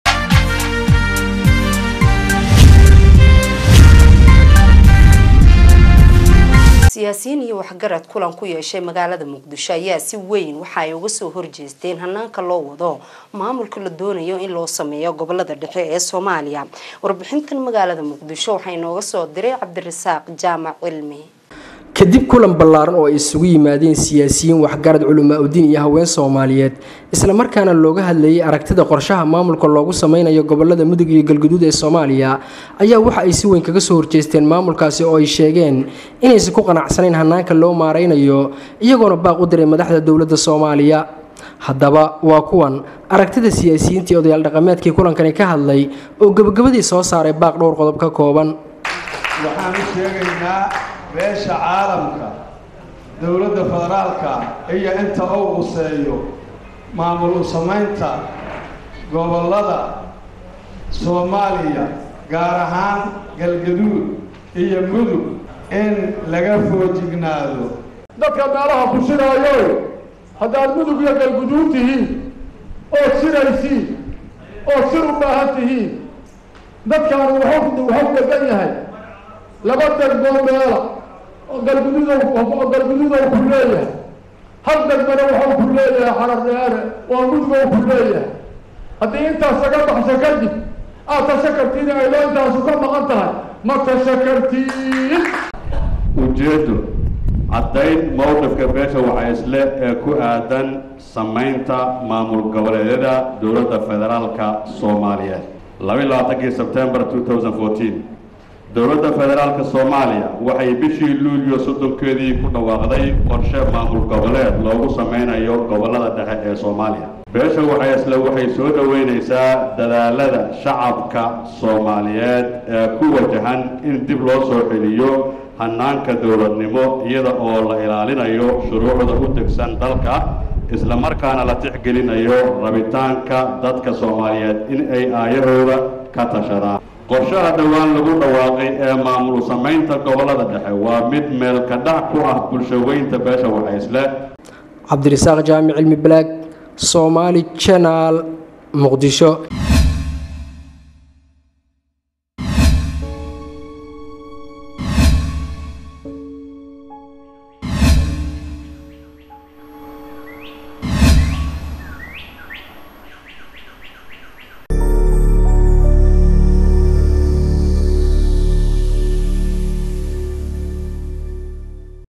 Siyaasiyiin Iyo Waxagarad Kulan Ku yeeshay Magaalada Muqdisho Oo si Weyn Kaga Soo Horjeestay Hanaan
Siyaasiyiin Iyo  Waxagarad Kulan Ku yeeshay Magaalada Muqdisho Oo si Weyn Kaga Soo Horjeestay Hanaanka  Loo Wado Maamul U sameeeynta Gobolada Dhexe ee Dalk Soomaaliya .